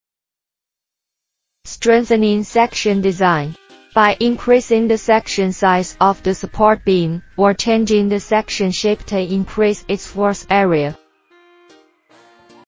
How to increase the section strength?（01）
The section area of the material is increased by increasing the section size, so as to improve the bending resistance, compressive resistance and tensile resistance of the material. Increasing the section can increase the load carrying capacity of the material.